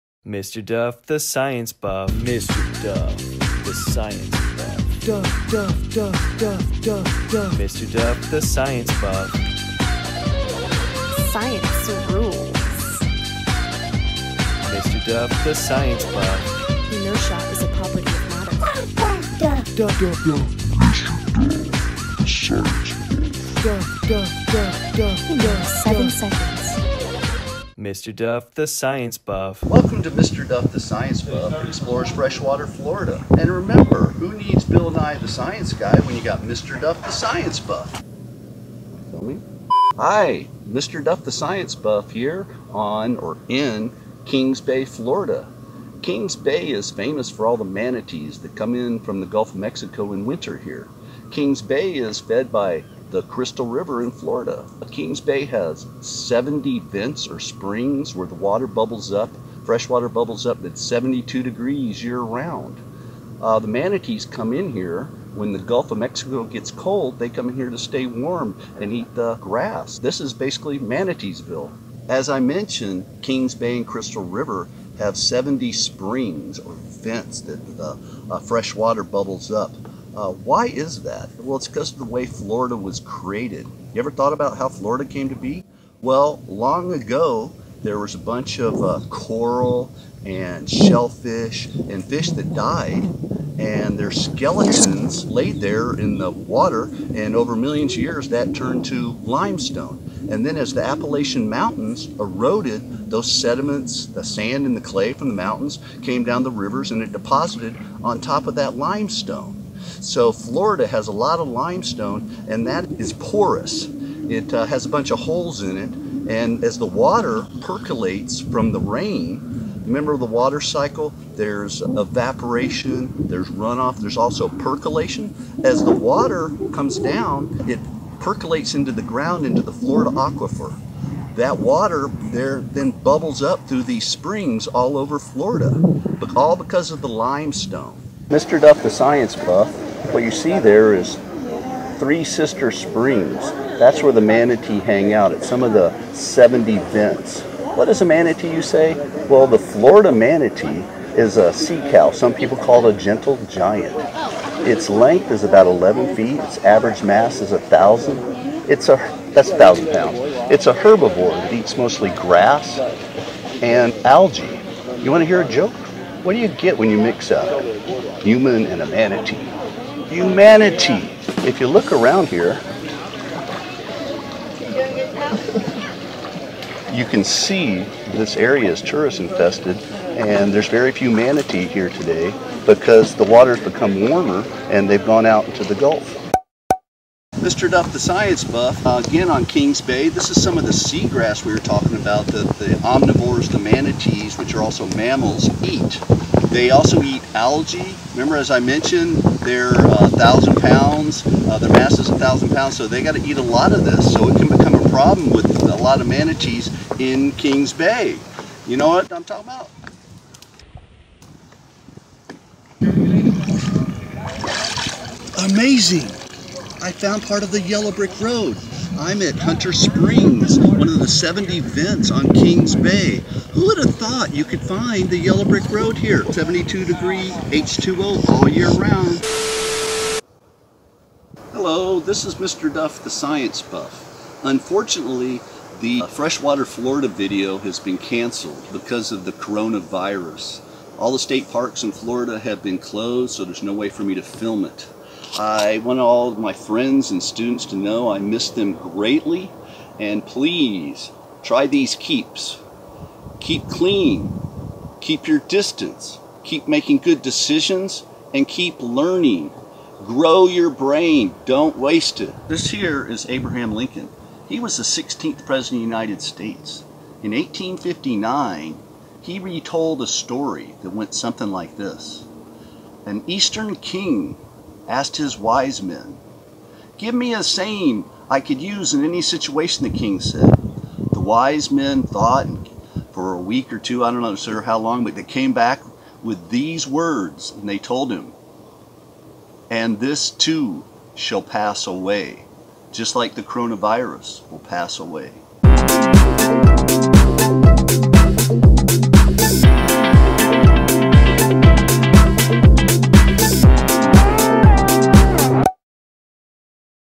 Mr Duph Explores Freshwater Florida
Unfortunately, our filming of this video was incomplete because of extenuating circumstances. However, we hope you enjoy regardless, and remember to practice the "keeps!"

Mr. Duph Explores Freshwater Florida covers the following science standards- Formation of Florida, manatees, manatee’s habitat, freshwater springs, vents, limestone, aquifer, erosion, sediments, percolation, deposition, water cycle, herbivores, mammals and Abraham Lincoln.
Filmed mostly in Crystal River Florida this video covers several elementary school science standards.